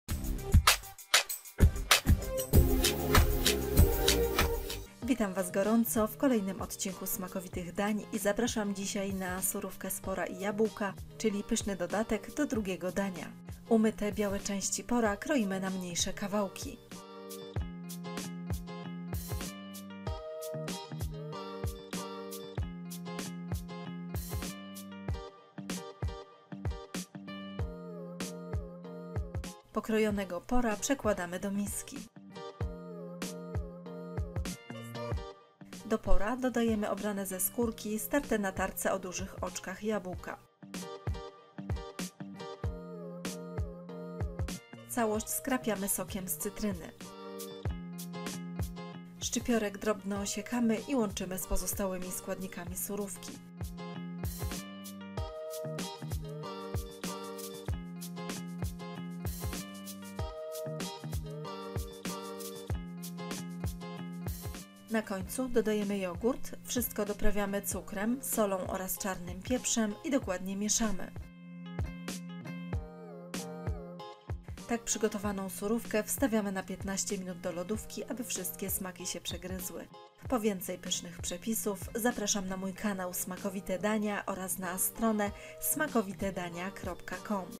Surówka z pora i jabłka - Jak zrobić - Smakowite Dania
Surówka z pora i jabłka to świetny dodatek do drugiego dania, przygotowuje się ją błyskawicznie, jest zdrowa, smaczna i bardzo prosta.

składniki na 4 porcje

2 duże pory ( białe części )

2 kwaskowate jabłka

sok z połowy cytryny

150 g jogurtu  naturalnego

1 pęczek szczypiorku

szczypta cukru kryształu

czarny pieprz

sól